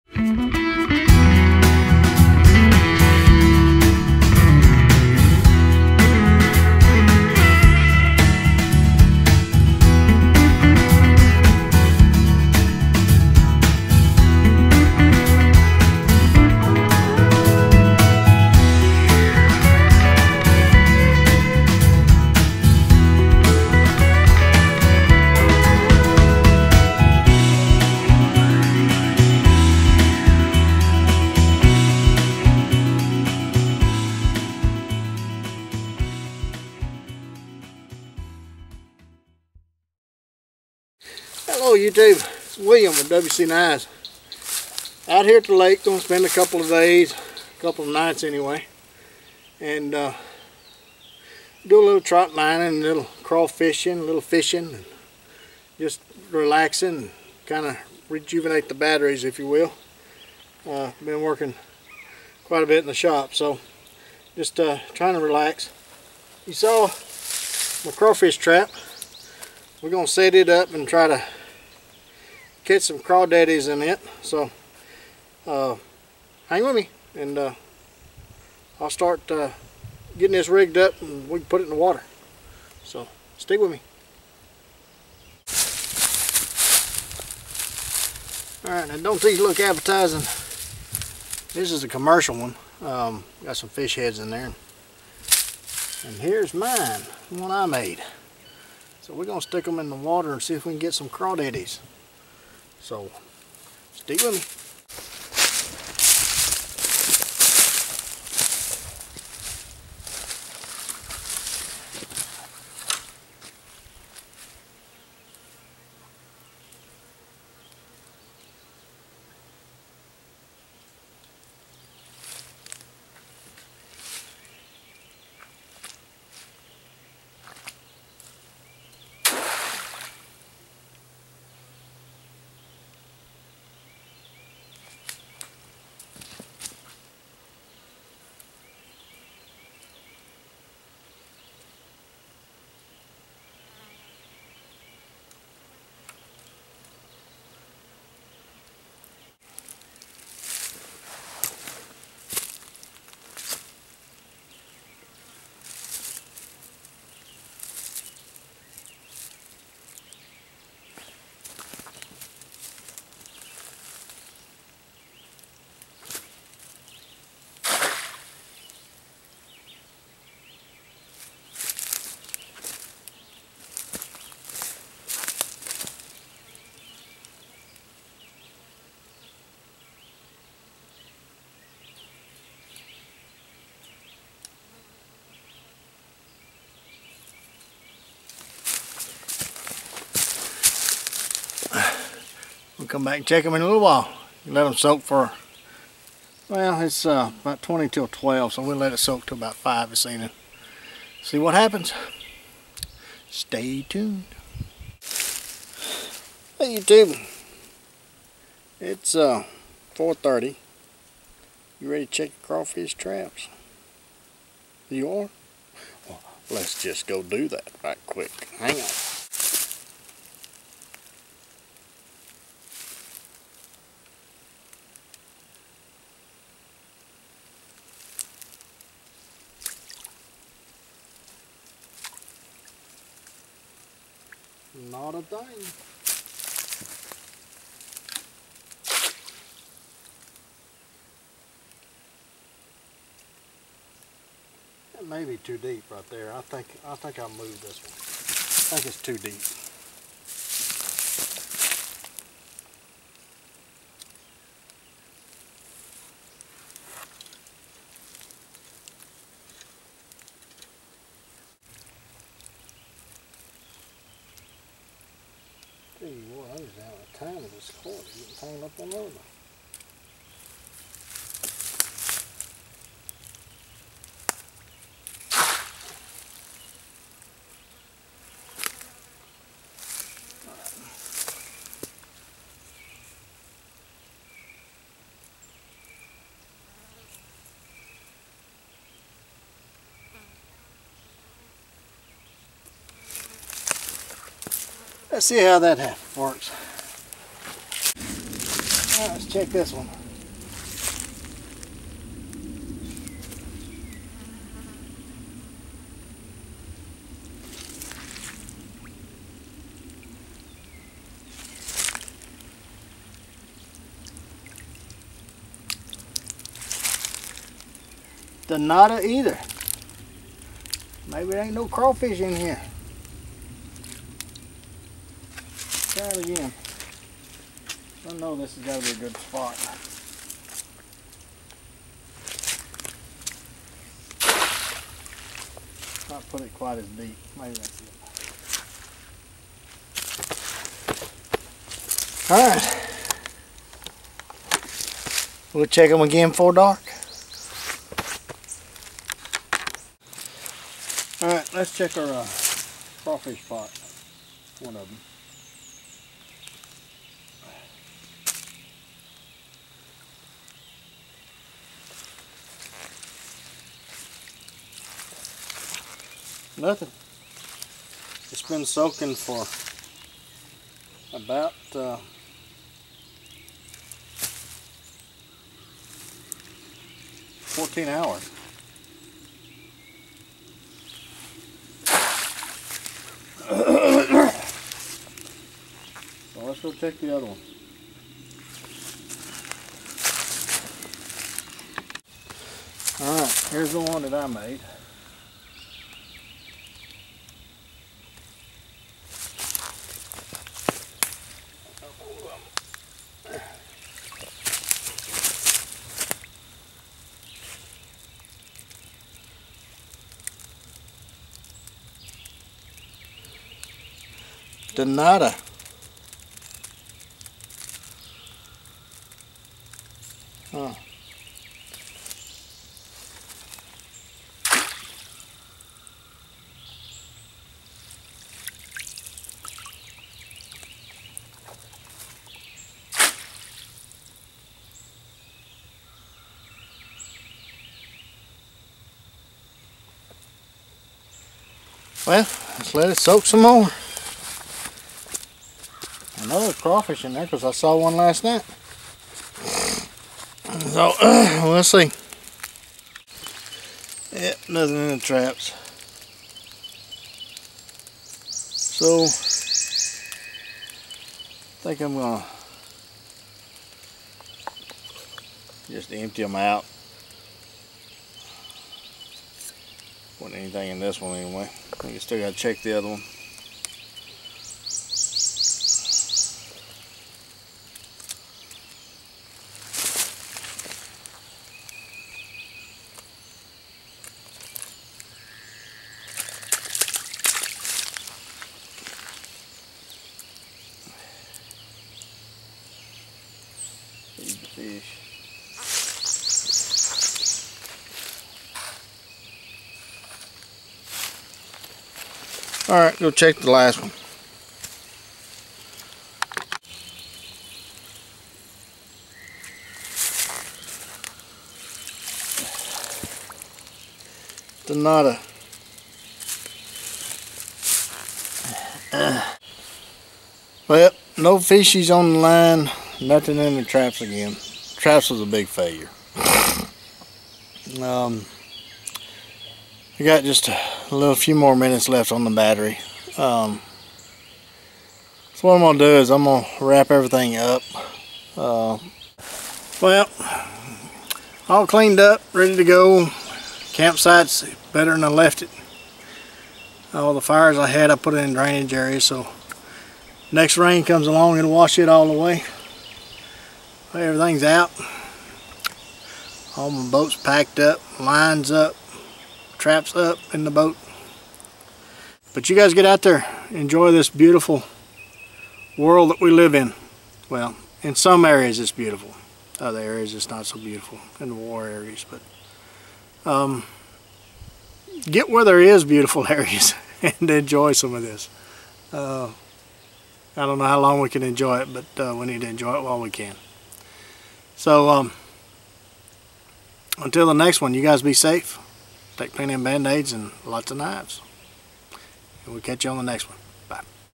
Setting and Checking the Crawfish Traps
No crawdaddy's but it was fun anyway. Will try different bait and location next time. Not giving up.